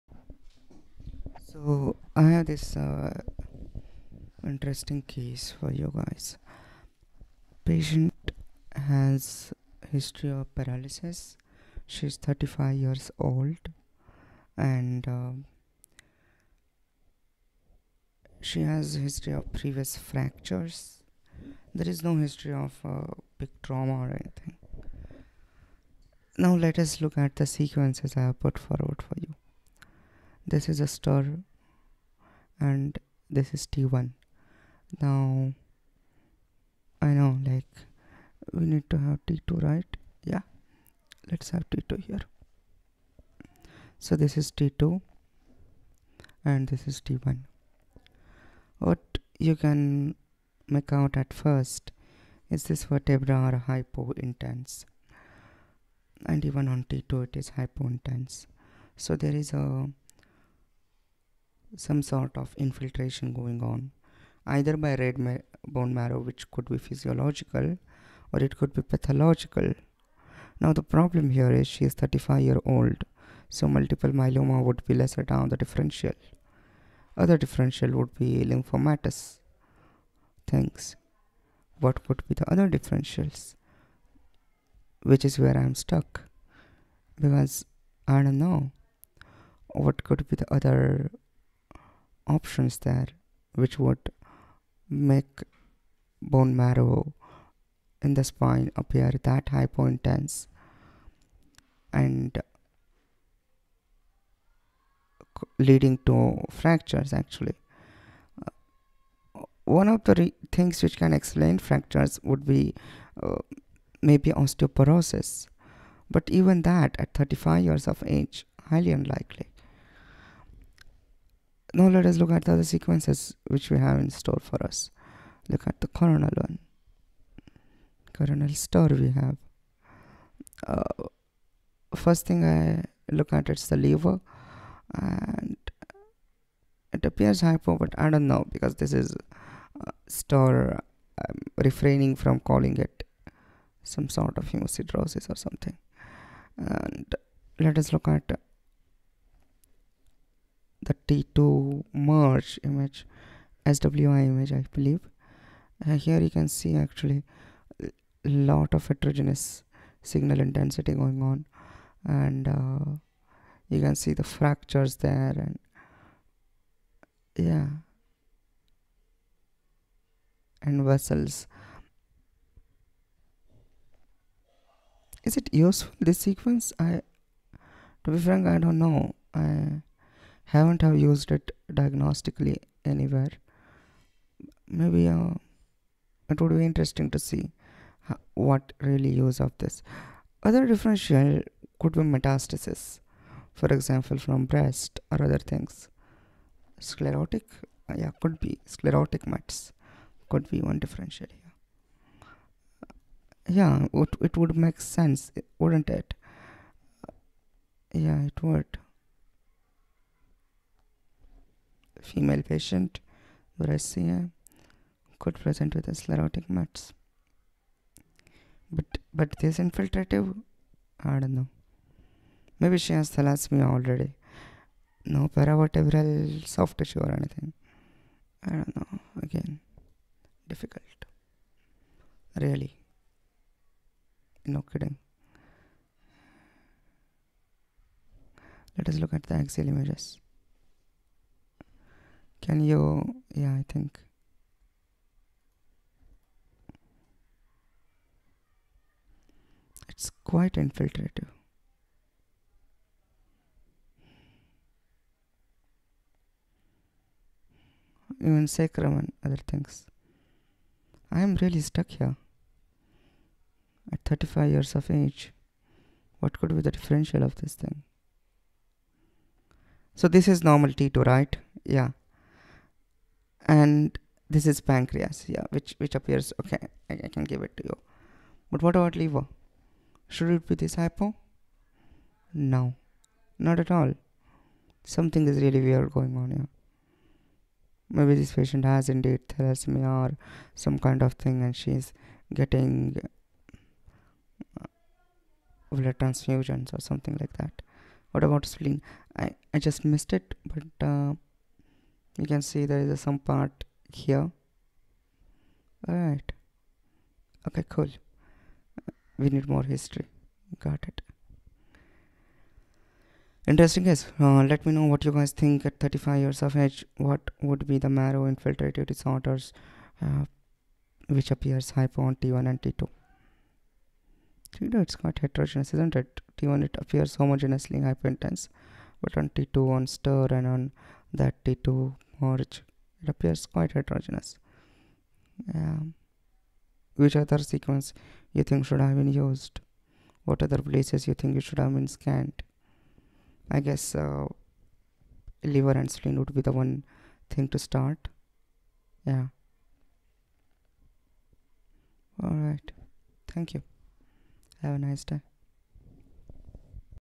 Bone marrow infiltration at 35 yrs
Bone marrow infiltration D/D metastasis, lymphoma, red marrow transformation